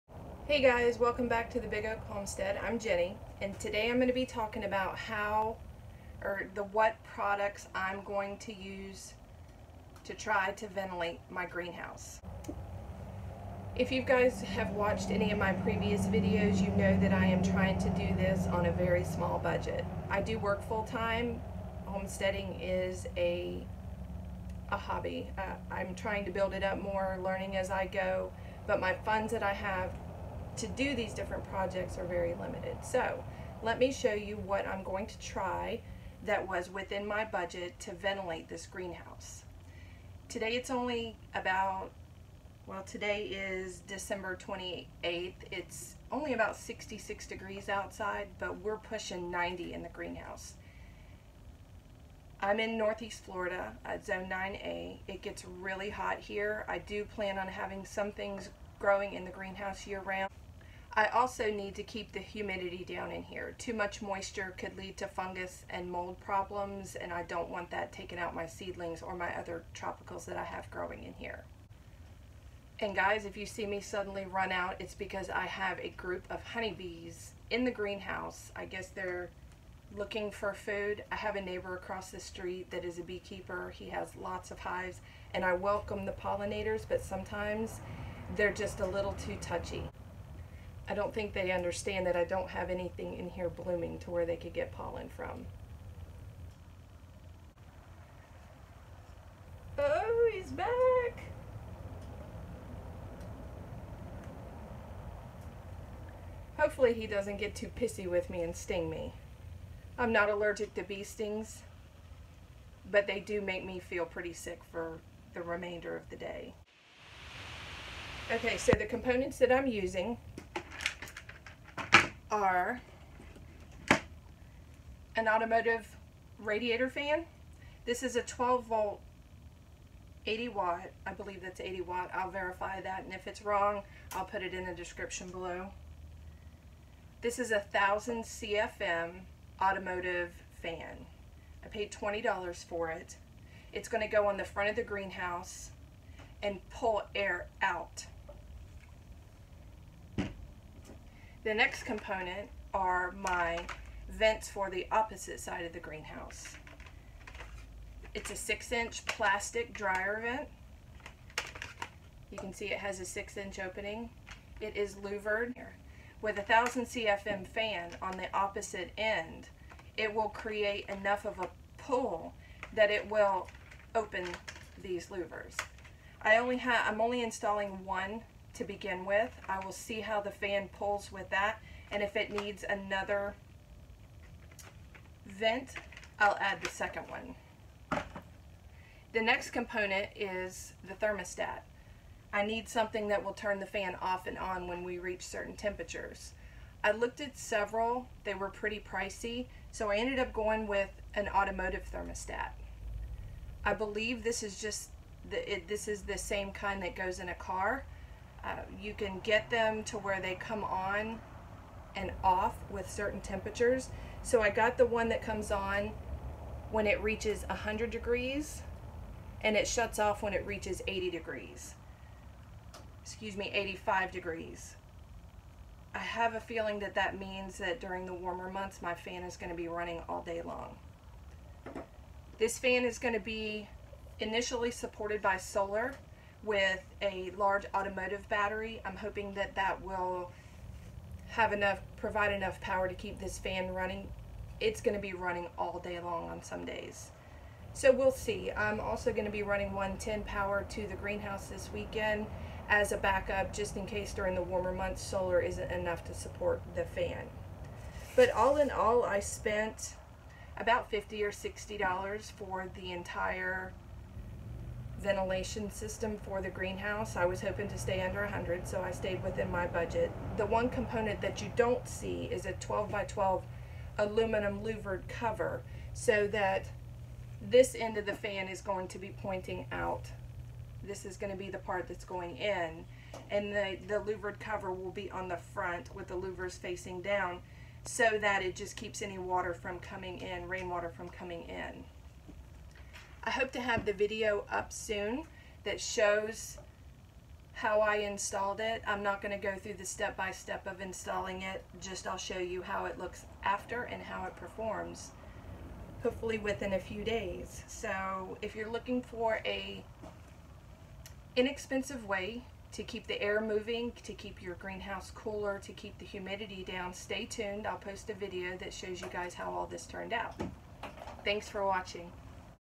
Can I ventilate my greenhouse with a radiator fan?
This video goes over what products I’m going to install to ventilate my greenhouse.

Universal Slim Fan Push Pull Electric Radiator Cooling Fans 12V 80W Engine Fan with Mount Kit （Diameter 8.27" Depth 2.56")

Lambro Industries 361W 6" Louver Vent Hood

Thermostat Switch - Circuit On At 100°F and Off At 85°F - Large Flange